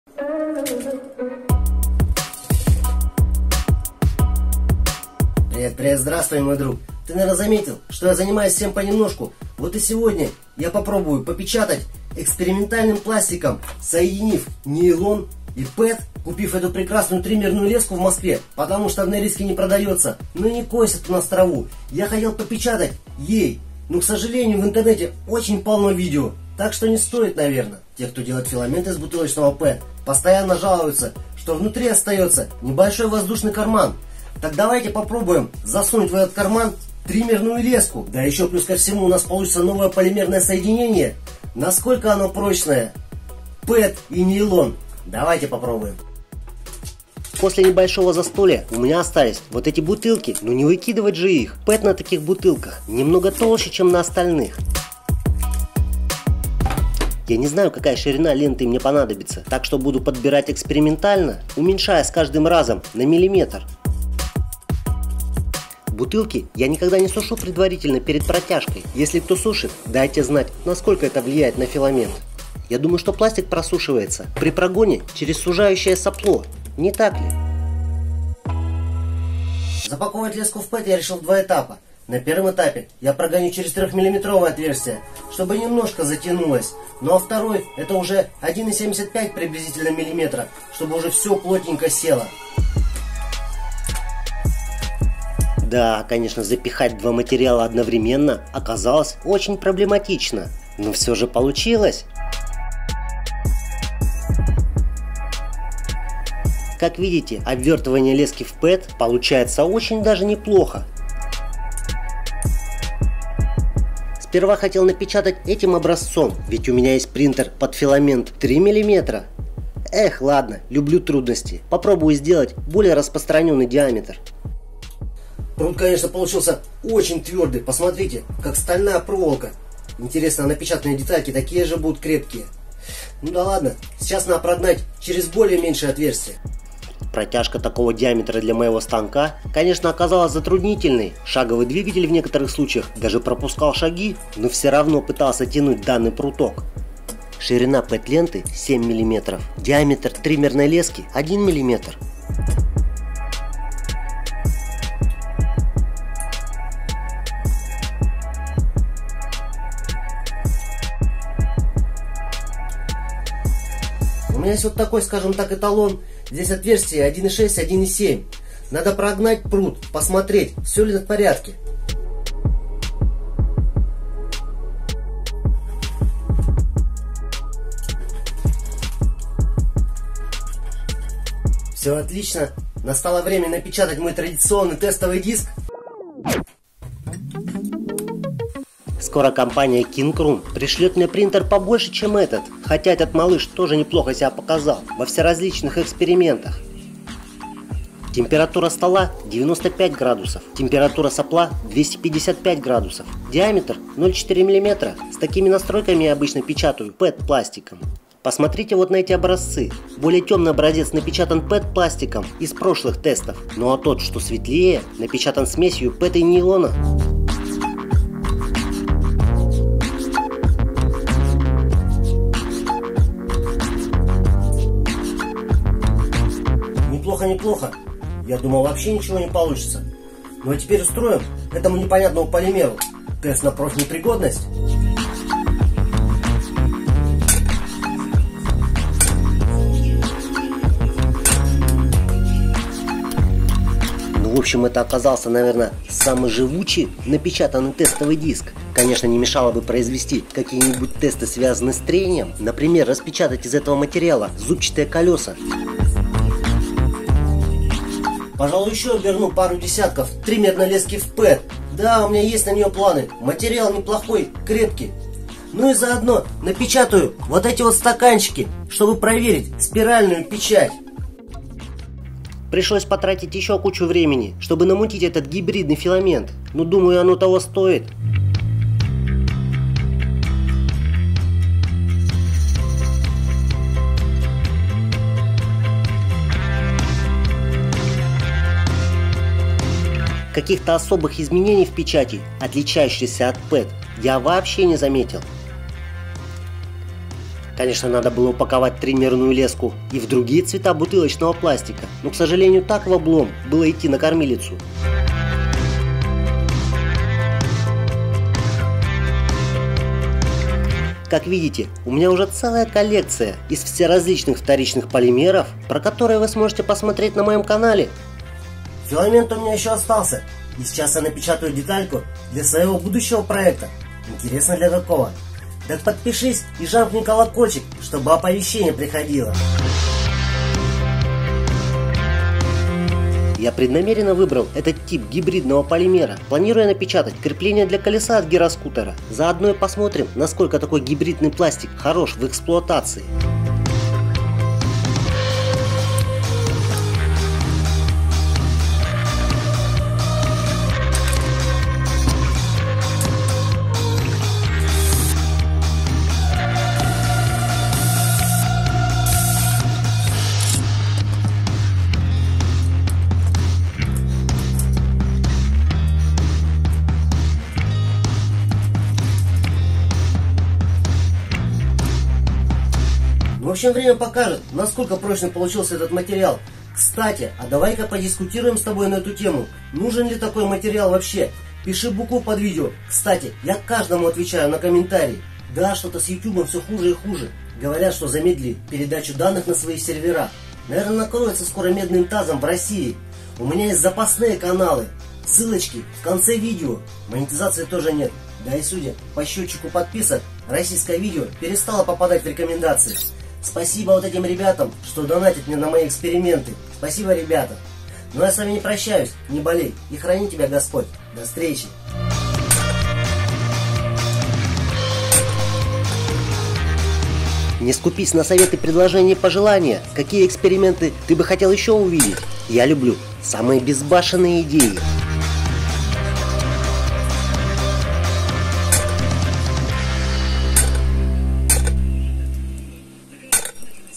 Эксперимент ПЭТ+НЕЙЛОН высокопрочный материал для неубиваемых деталей, что из этого получится?
Здравствуй Друг !
Сегодня я попробую печатать экспериментальным пластиком,  PET+NYLON.Те кто делает филамент из бутылочного PET постоянно жалуются, что внутри остается воздушный карман.  Давайте попробуем засунуть NYLON в центр филамента. Насколько  прочный PET+NYLON? давайте попробуем.
Эксперимент, смешать ПЭТ+НЕЙЛОН что из этого получится?
После застолья у меня остались бутылки. Ну не выкидывать же их. Пластик на таких бутылках немного толще чем на остальных.
Бутылки я не слушал предварительно перед протяжкой, думаю что пластик просушится при прогоне через сужающие сопло. Ширина ленты 7мм., диаметр лески 1мм.
Запаковывать NYLON в PET я буду в два этапа. На первом этапе сделаю ~ 3мм., чтобы немножко затянулось. Второй это уже ~1.75 миллиметра. Как видите обвертывание NYLON в PET получается очень даже неплохо. Филамент очень твердый, как стальная проволока.
Настало время напечатать традиционный тестовый диск. Температура стола 90°, сопла 255° диаметр 0.4мм с такими настройками я обычно печатаю PETом.
Материал неплохой, крепкий. Напечатаю  стаканчик, чтобы проверить спиральный печаль . Каких-то особых изменений в печати отличающийся от PET я не заметил.
Надо было  упаковать NYLON и в другие цвета бутылочного пластика. Как видите, у меня уже целая коллекция из различных вторичных полимеров..